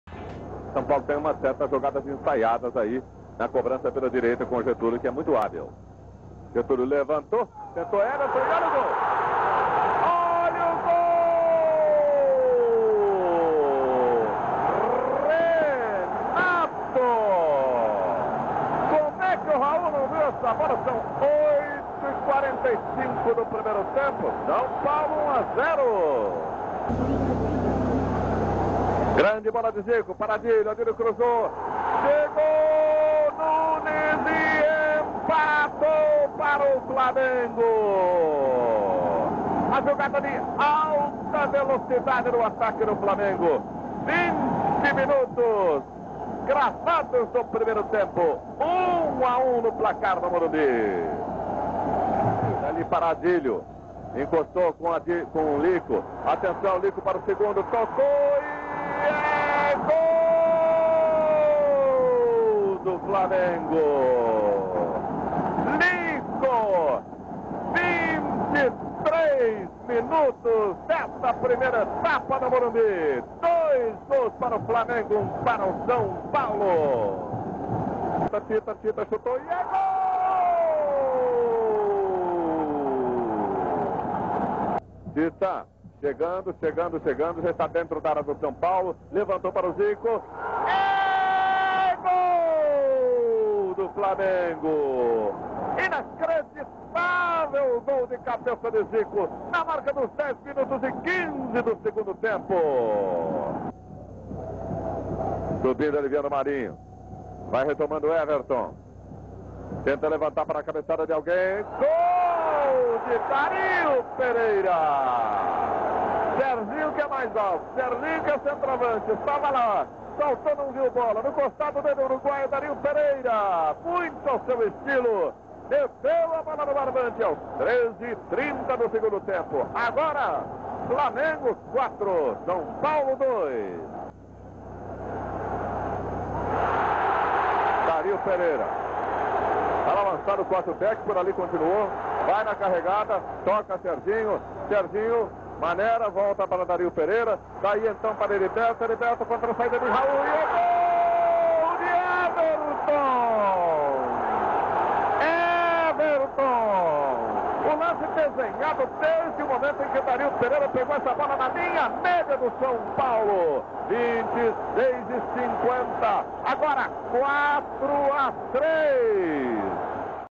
São Paulo 3 x 4 Flamengo (Campeonato Brasileiro 1982)
Gols da partida realizada no Estádio do Morumbi, em São Paulo (SP).

Narração: Fernando Solera